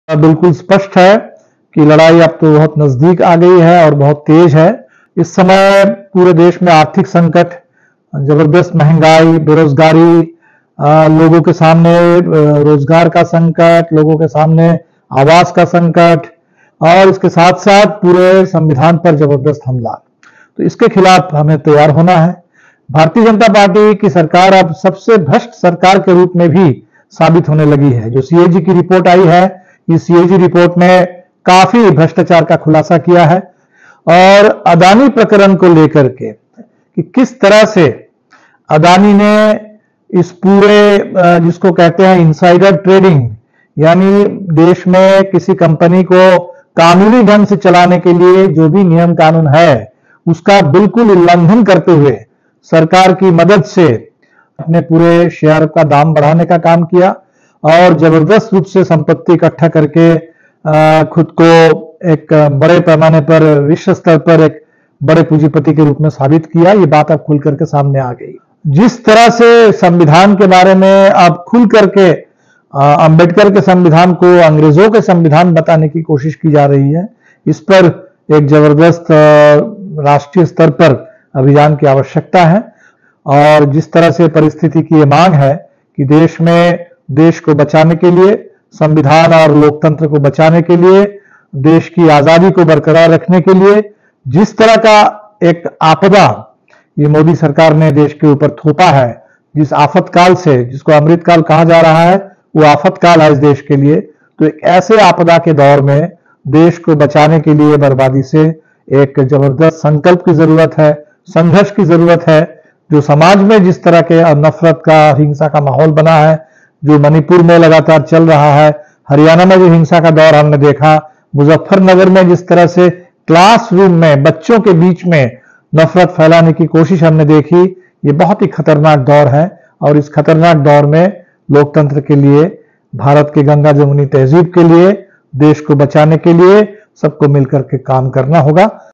आपदाओं की जंजीर को काटने और भारत को फासीवादी विनाश से मुक्त करने के लिए आगे बढ़े
अब समय आ गया है कि भारत, जीवन के सभी मोर्चों पर मोदी शासन द्वारा थोपी जा रही आपदाओं की जंजीर को काटने और भारत को फासीवादी विनाश से मुक्त करने के लिए आजादी हासिल करने वाली शानदार उपनिवेशवाद-विरोधी संघर्ष से ऊर्जा और प्रेरणा ले. - Dipankar Bhattacharya